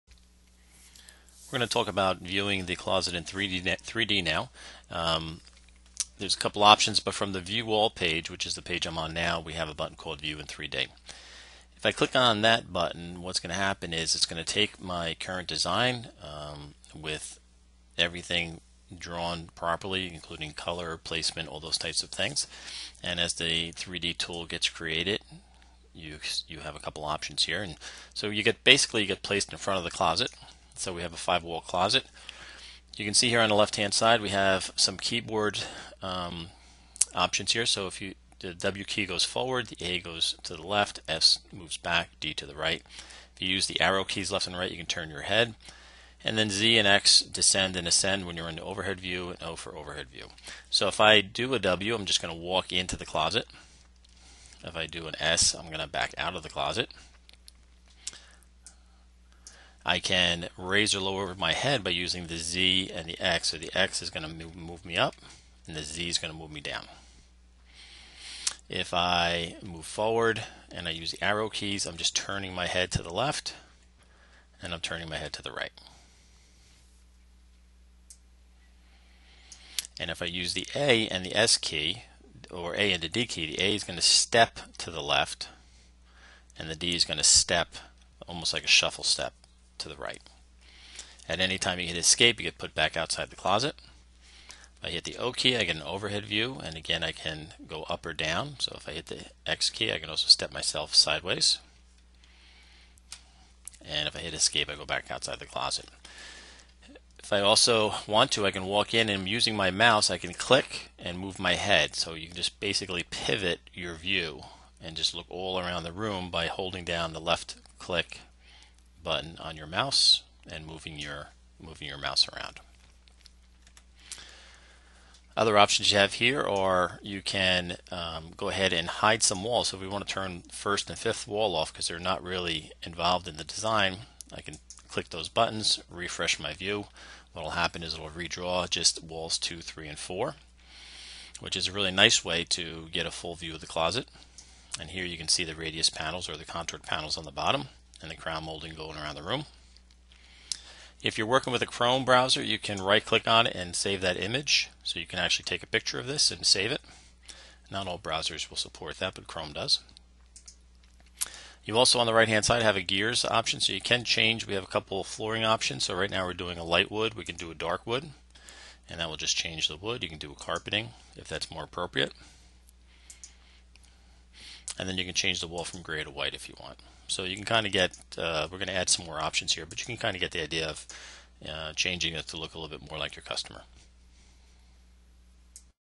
Interactive 3D Viewing
This video shows you how to use the Interactive 3D View.  The options explained include:
- Moving around the closet
- Overhead view
- Reset view using ESC key
- Turn walls on/off
- Change Floor color
- Change Wall Color